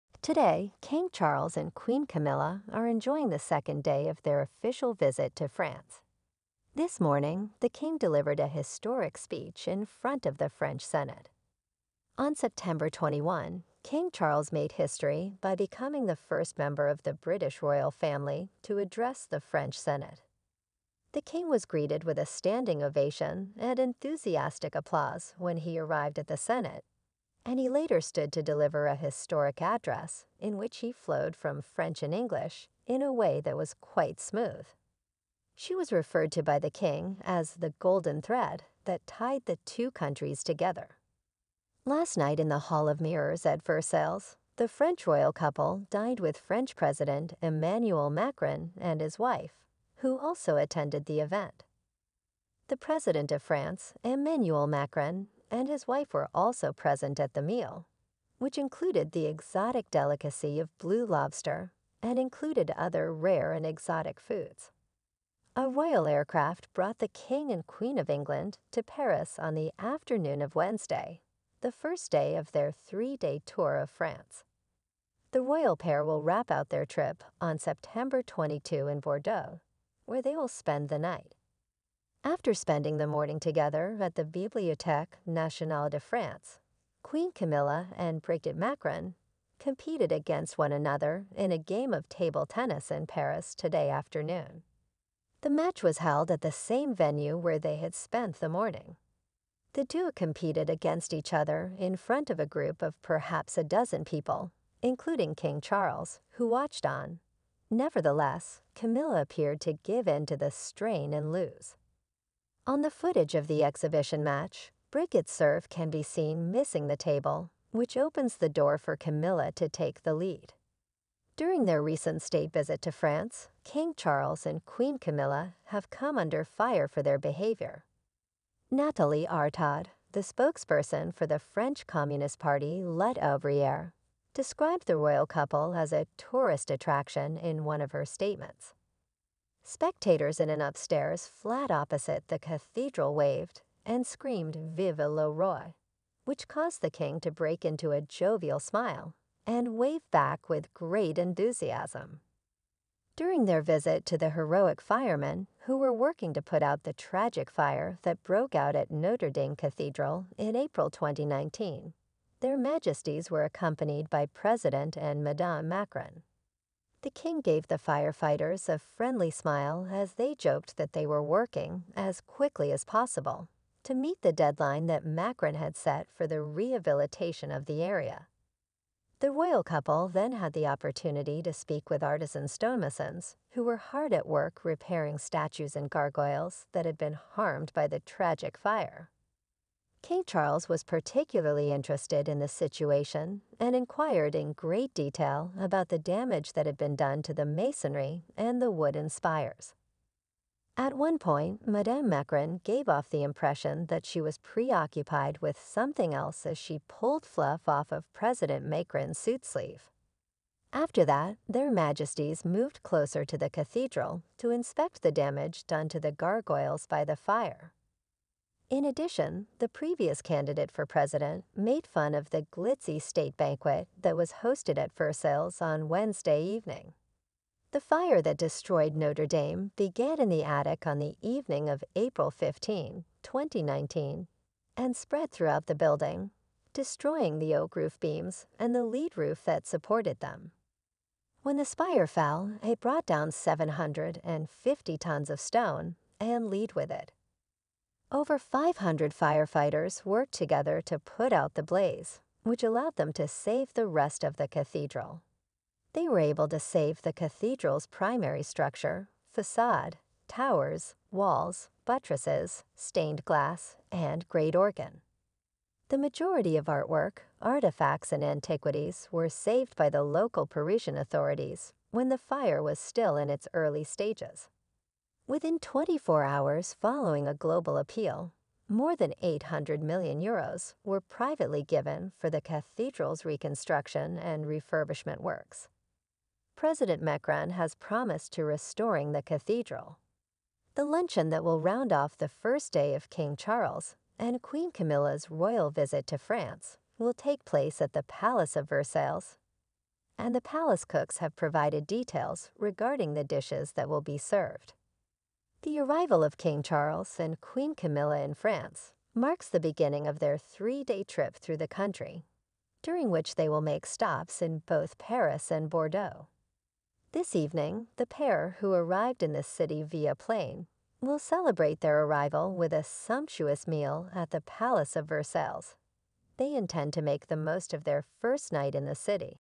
LIVE coverage of the King and Queen's journey, during which Prince Charles made history by..
LIVE coverage of the King and Queen's journey, during which Prince Charles made history by becoming the first British royal to address the French Senate.